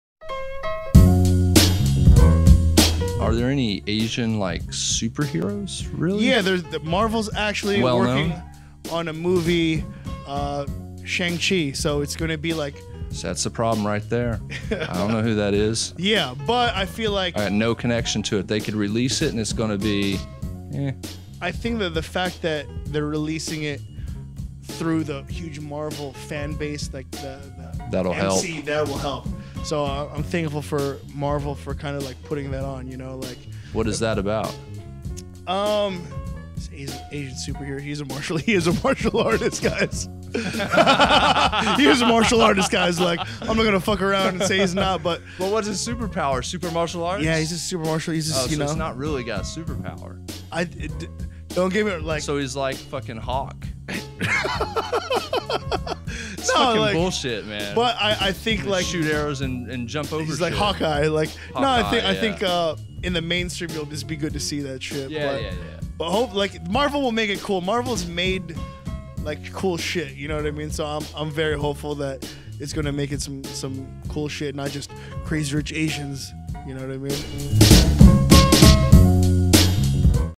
Asian Superheroes
How will Marvel's newest superhero, Shang-Chi, be received by popular culture?